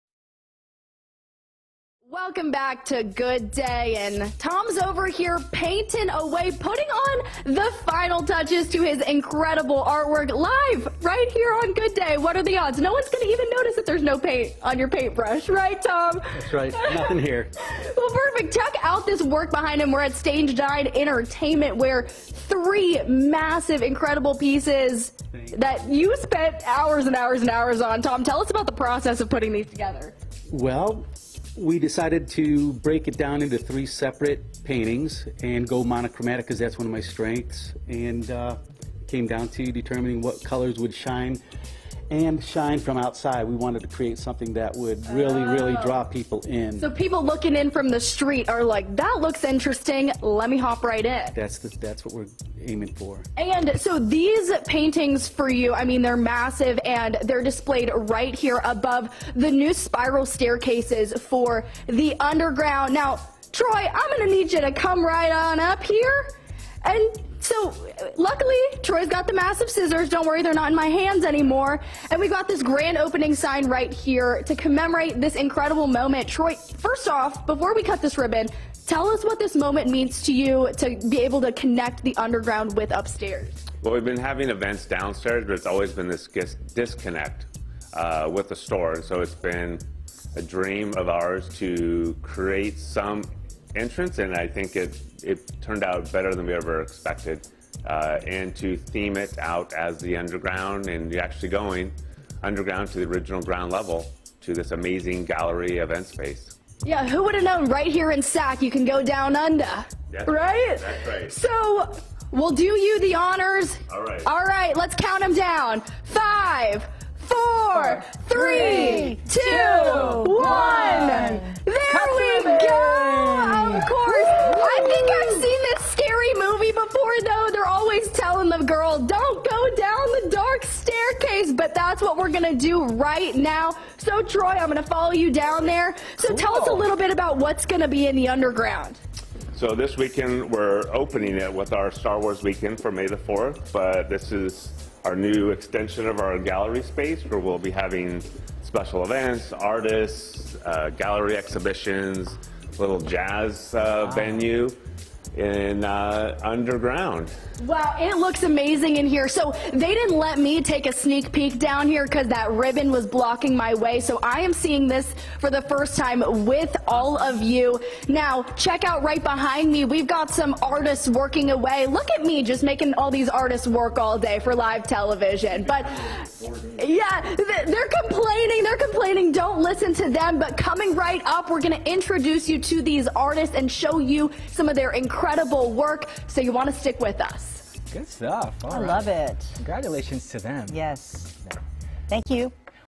Unveiling "The Underground"
Stage Nine Entertainment invites the community to welcome two Thomas Kinkade Studios Artists - Monte Moore and Brian Rood - for a special "Star Wars™ Weekend" on May 6-7, 2023. Stage Nine will also unveil a new spiral staircase that takes guests from The Vault to a completely remodeled The Underground event space just below the popular retail store and art gallery.